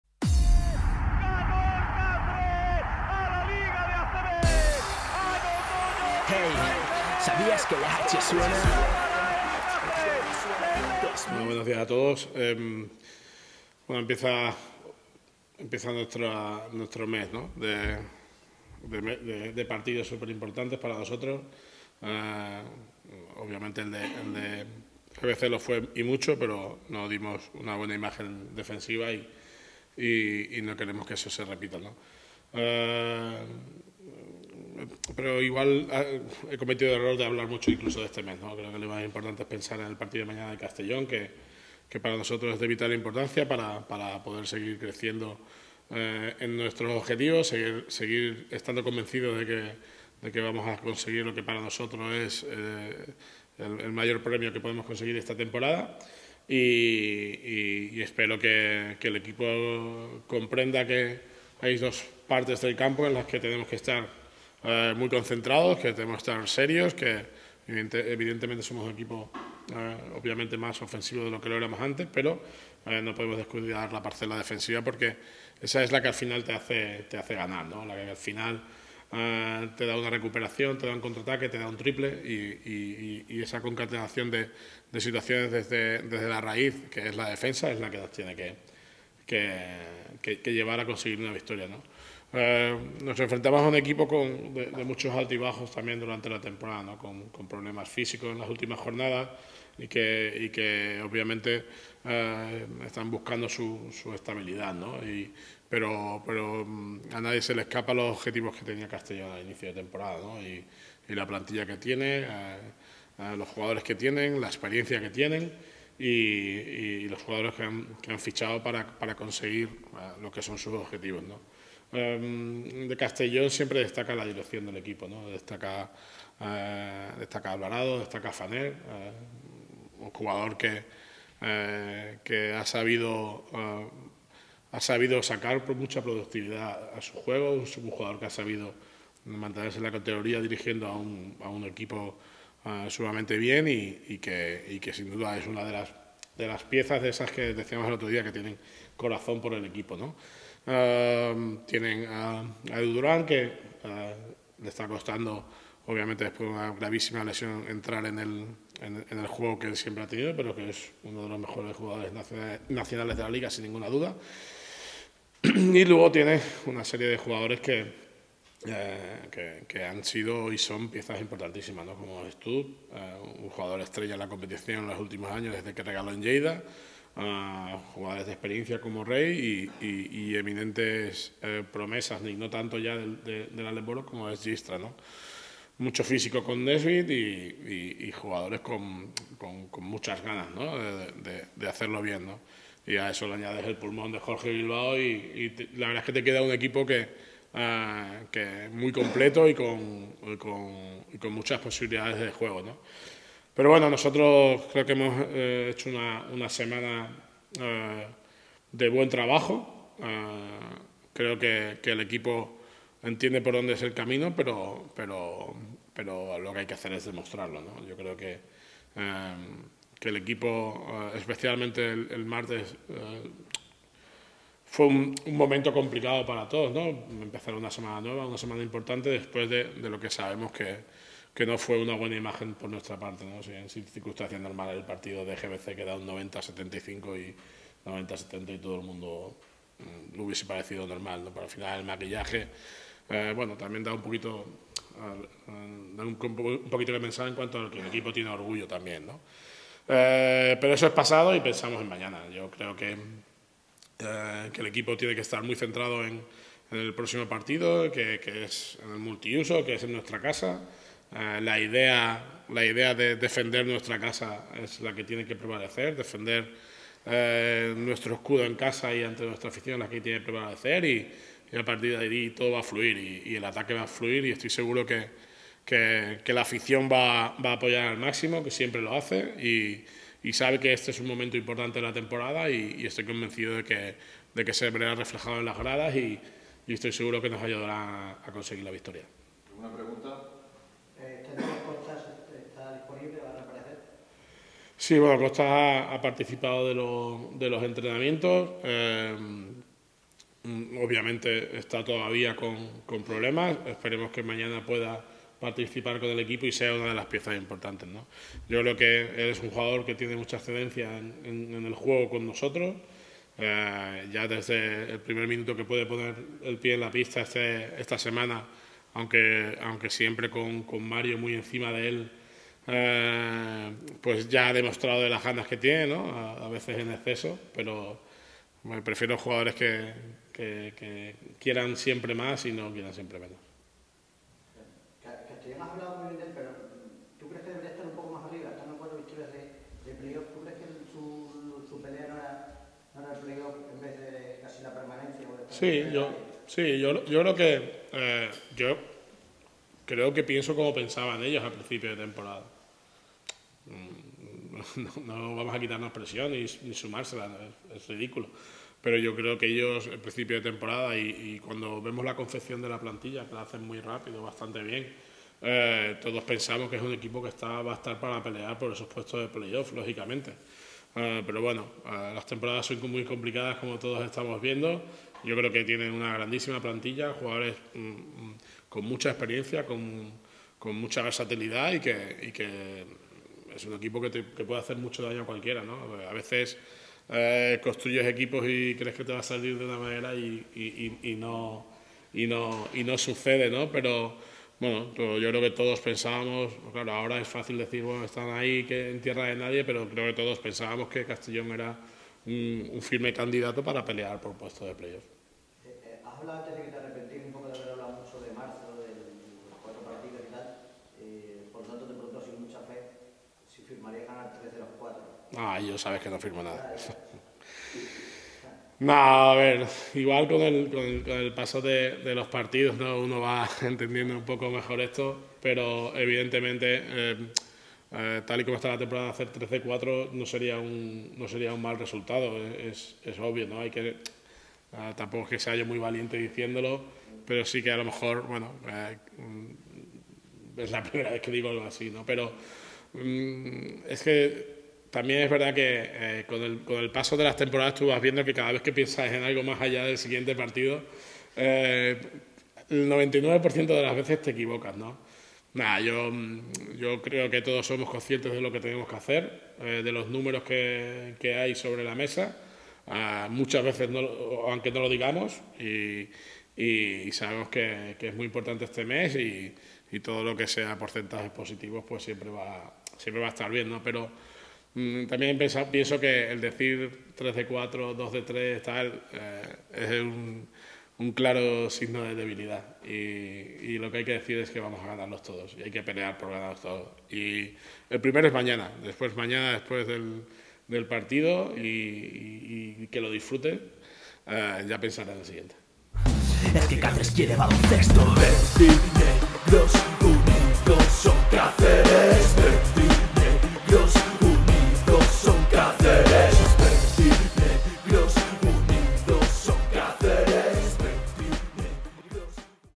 T22/23 - Prev. J23 LEB Oro- Cáceres Patrimonio de la Humanidad - TAU Castelló
Rueda de prensa de Roberto Blanco, entrenador del Cáceres Patrimonio de la Humanidad, previa al partido contra TAU Castelló de la jornada 23 de LEB Oro este viernes, 10 de marzo, a las 20:45h. en el Multiusos.